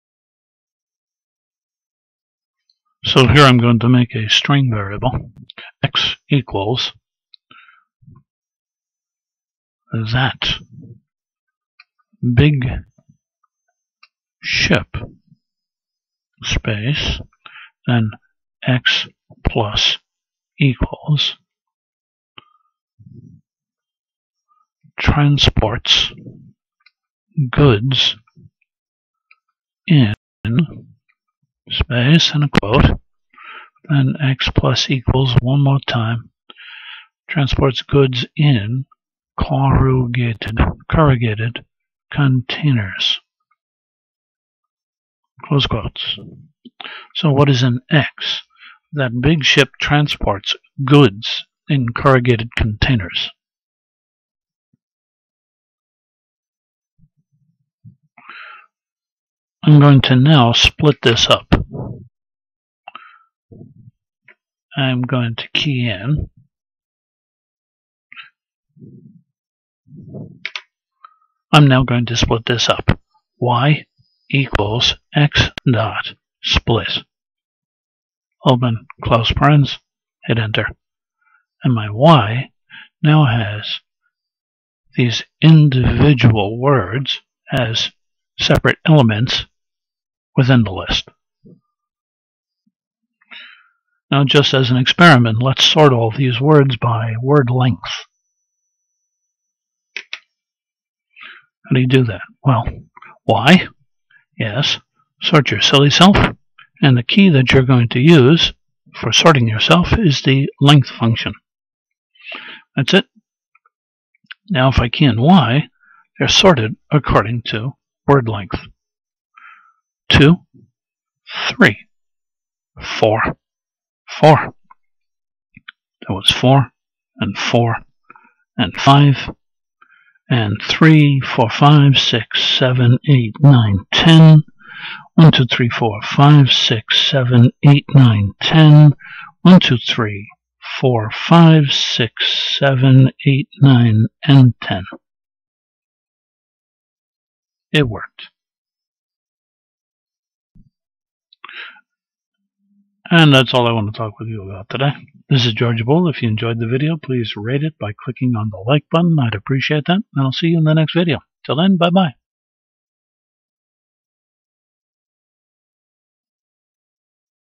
Python Strings: Splits & Sorts by Word Length
Sorting in Python post splitting of string data is demo'd